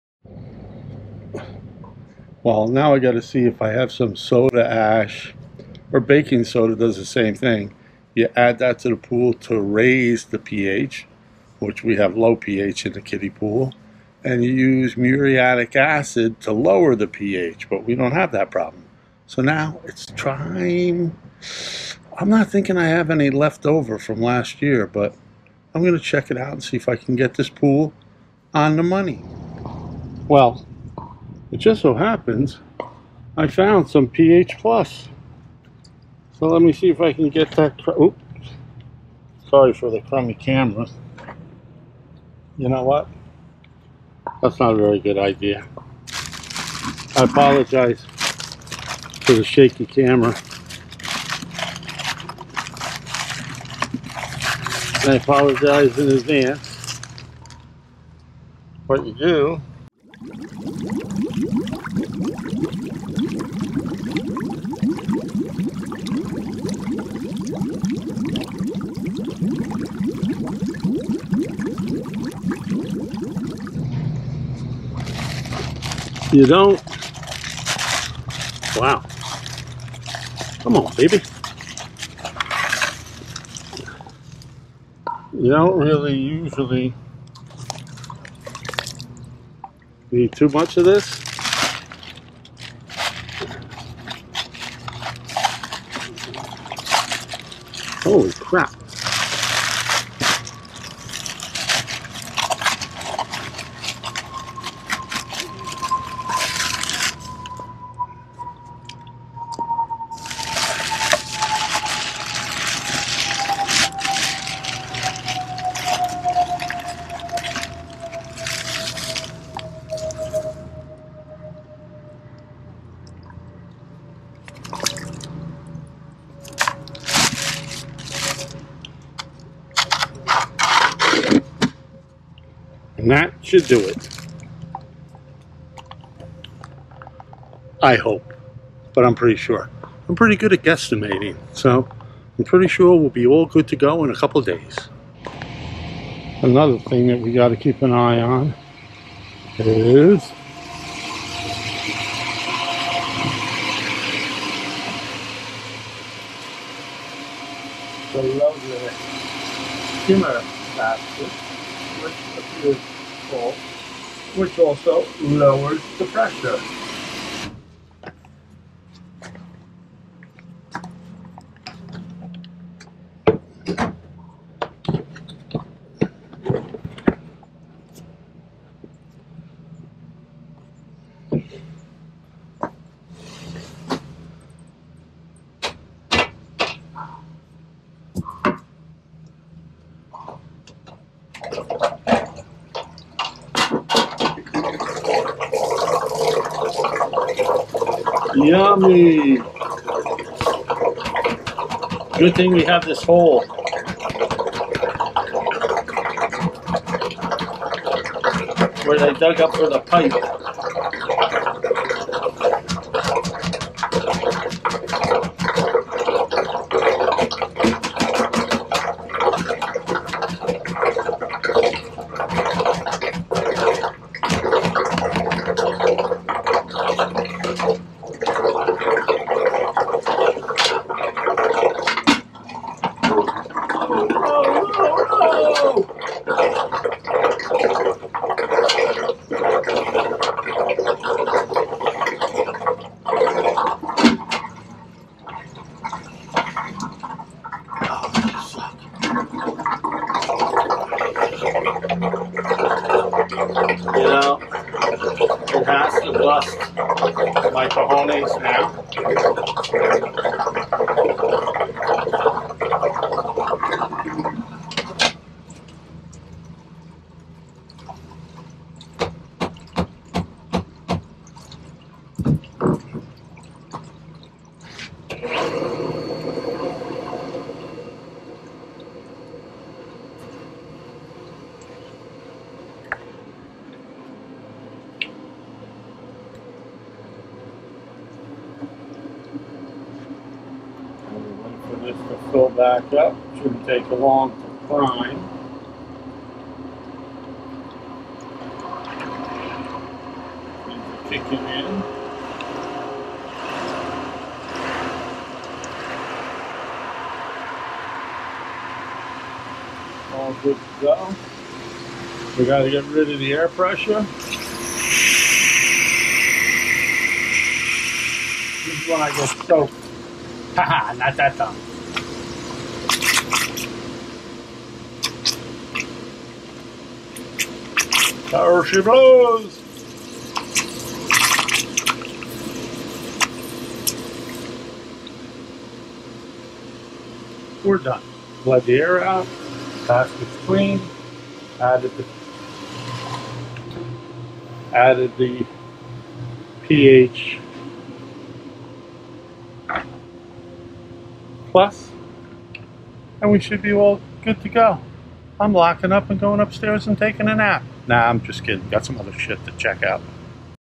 PH Adjustment Raising Your Pools PH
Just doing a little maintenance on the kiddy pool. Adjusting the PH and cleaning the filter basket.